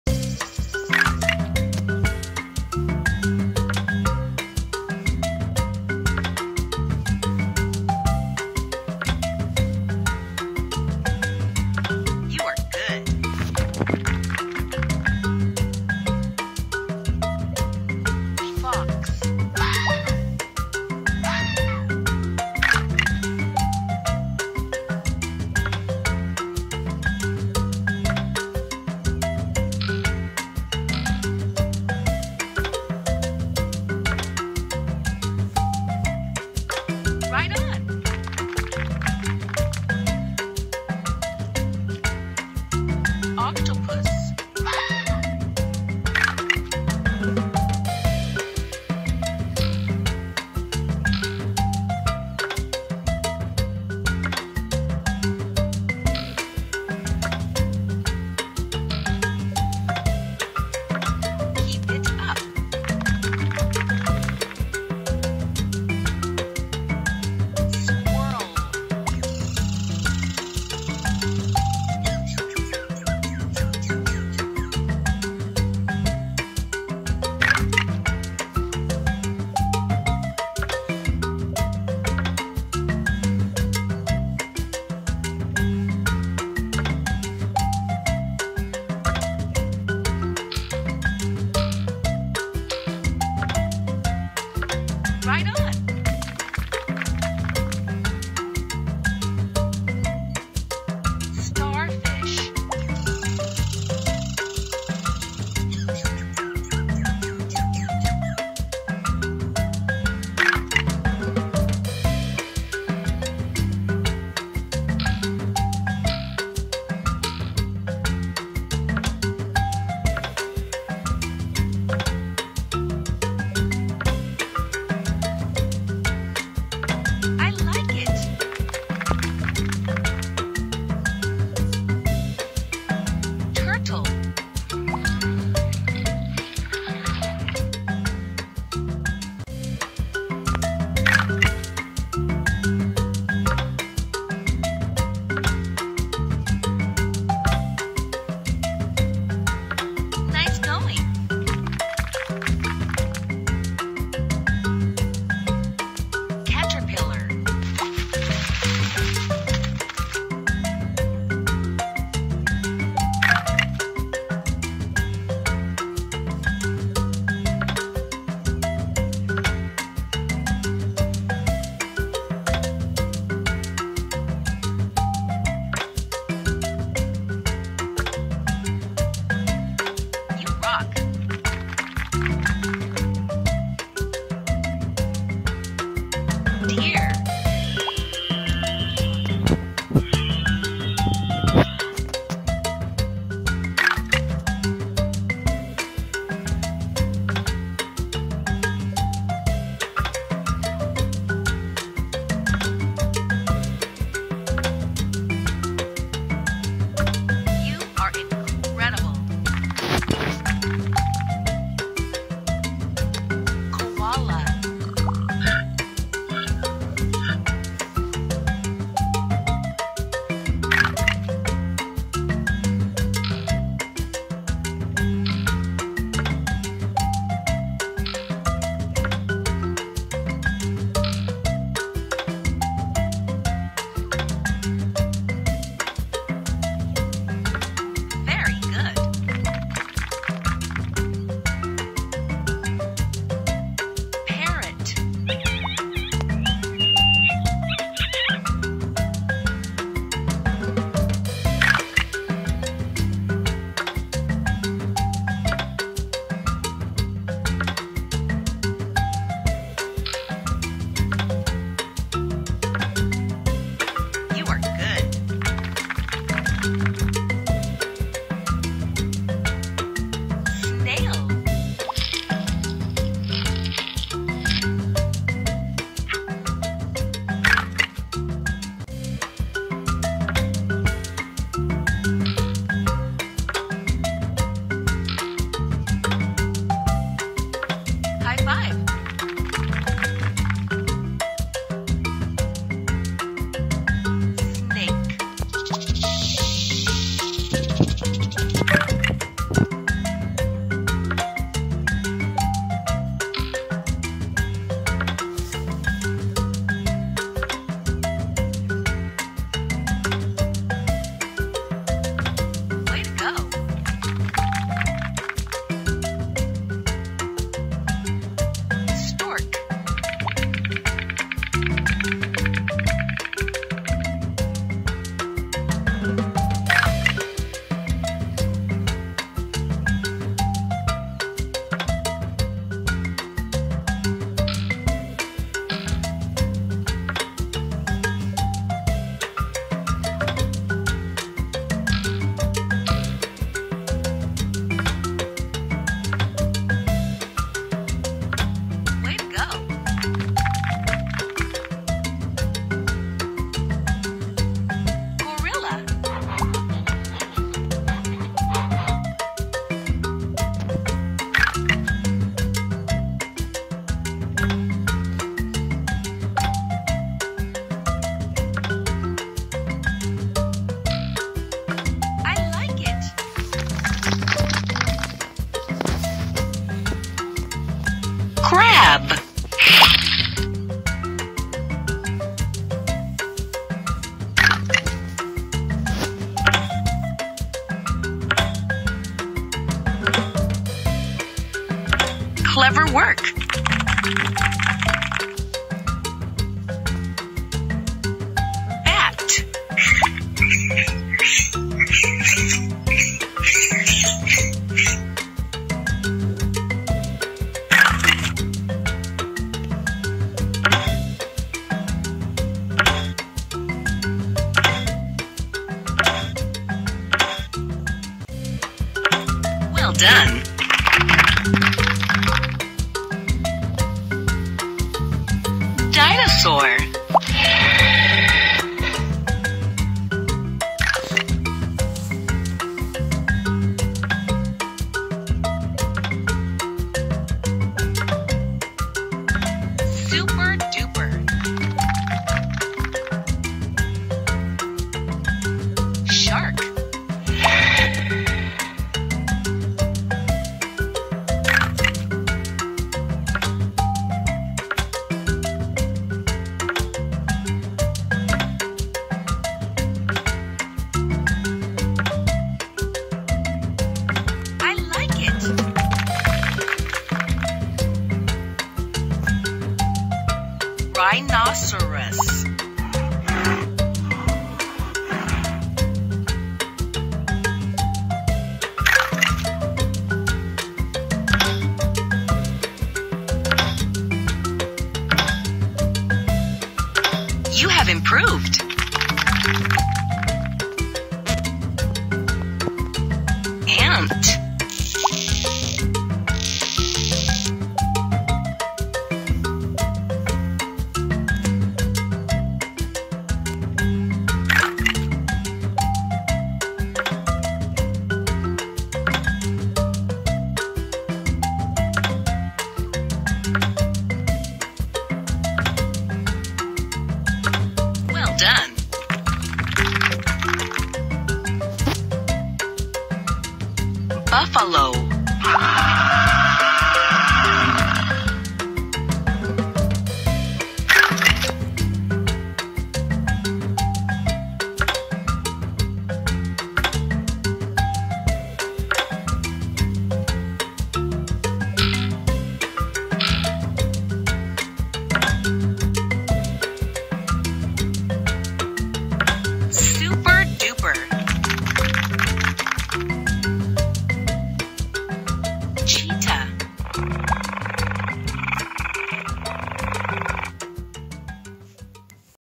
PUZZLES FOR CHILDREN. We add fox, squirrel, turtle, caterpillar, parrot, snake, gorilla hippopotamus
With this game you can start learning English.  Your children will not only be able to get distracted by watching how puzzles with animals and equipment are assembled, but also at the same time memorize the names of animals and objects in English.

Meet wild and domestic animals in these fun puzzle games for toddlers! These matching games will help you match figures to solve puzzles that reveal animals you might see in zoos, villages, farms! You will be able to see pigs, geese, monkeys, rams and many more in this game! These fun interactive games will entertain you regularly every day.